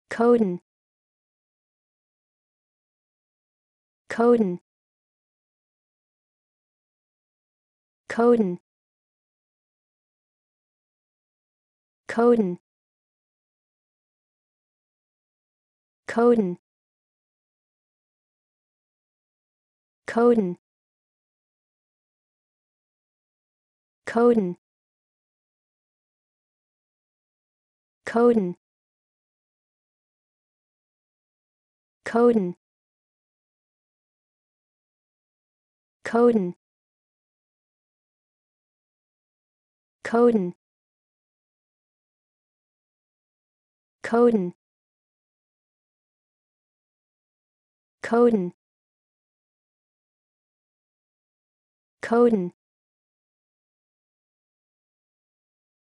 Como dizer 'codão' em ingles?
Como dizer codão em Ingles? Como se pronuncia codon? How to pronounce codon?